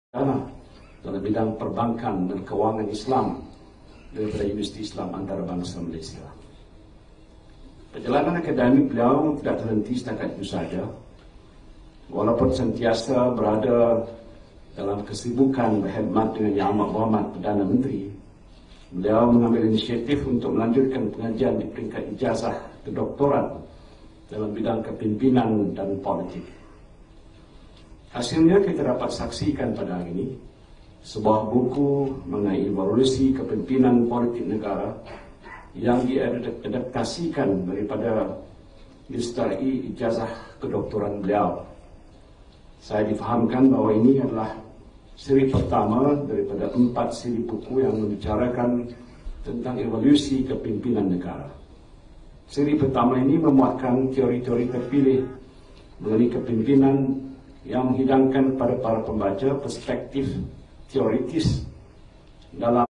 Professor Dato Wira Dr. Khairil Annas Jusoh - Book Launch 1
Portion of video - Book launch of Professor Dato Wira Khairil Annas Jusoh, author of the book Evolution of Leadership. Dato Wira Khairil Annas Jusoh